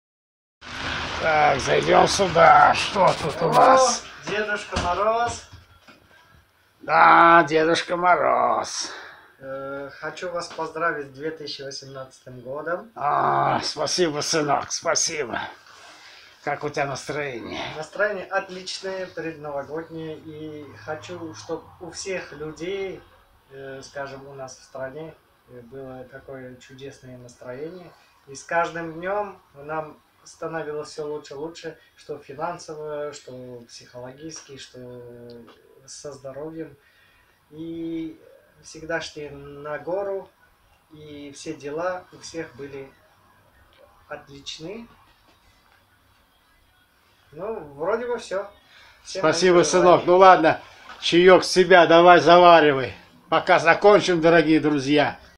Дедушка Мороз в Бавлах!!#
случайно дедушка зашёл в чайною лавку